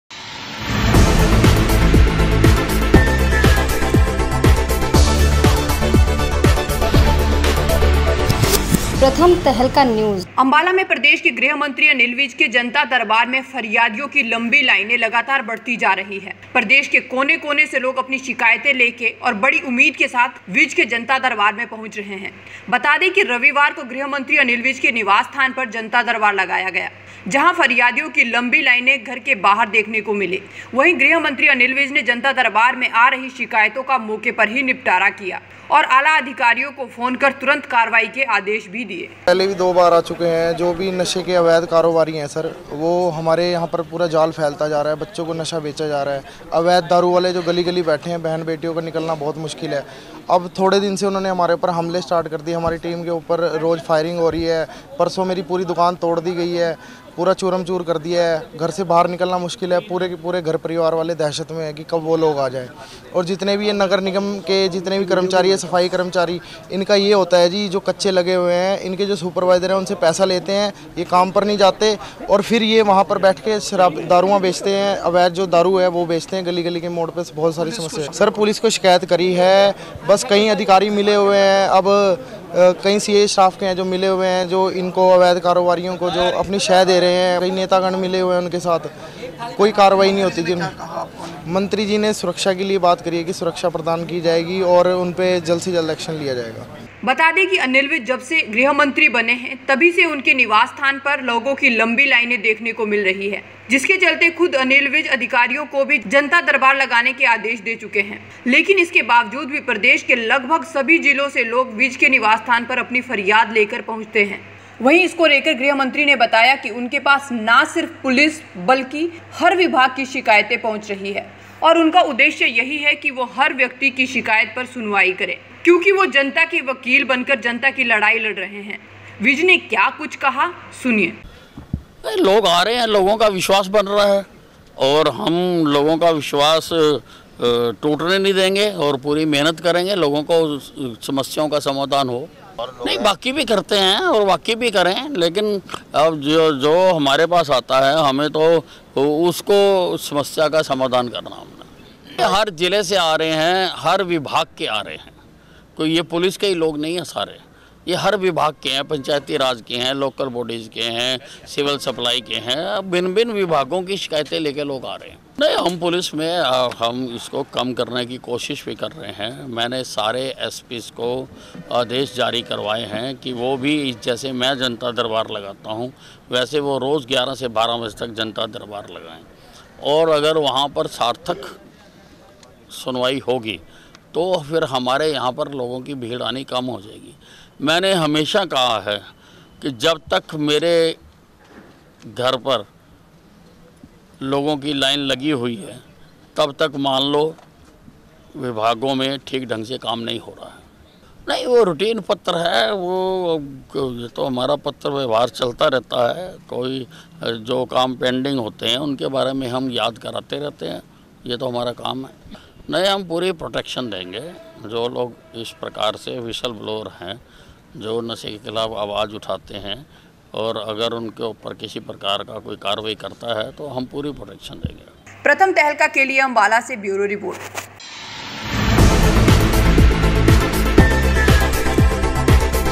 गृह मंत्री के जनता दरबार में दिखी फरियादियों की लाईनें, जनता के वकील बनकर लड़ रहे जनता की लड़ाई-विज
गृह मंत्री के जनता दरबार में दिखी फरियादियों की लंबी लाईनें, जनता के वकील बनकर लड़ रहे जनता की लड़ाई-विज